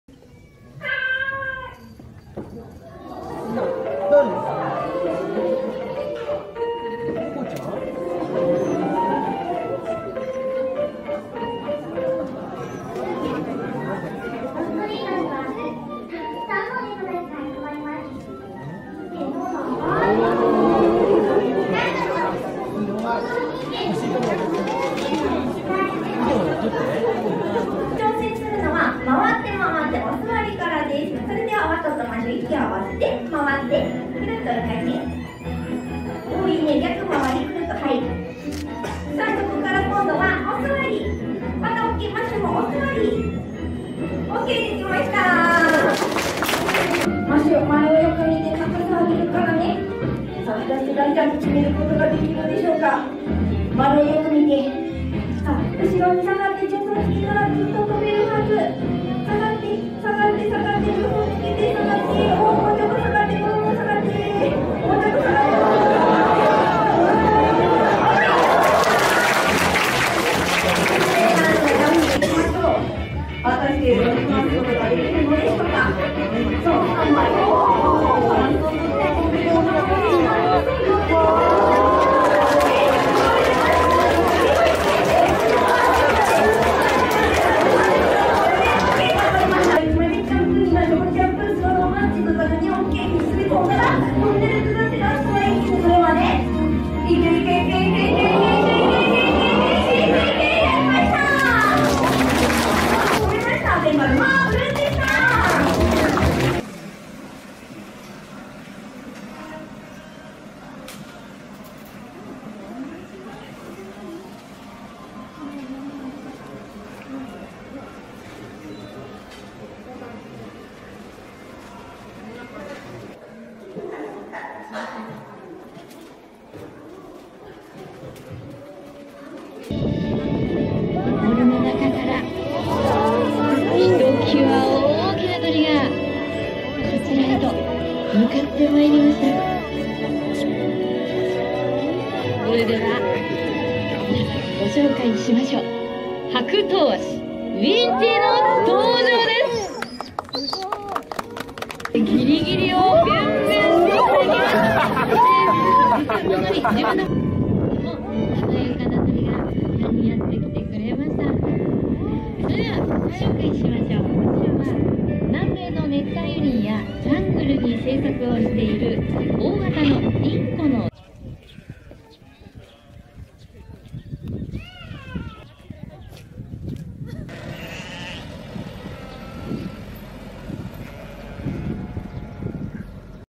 CAT SHOW(Circus) near Tokyo, Japan
cute cats perform on the stage at Tochigi, near Tokyo.
takes 3 hours from Tokyo by car or train.
zoo name is Nasu Animal Kingdom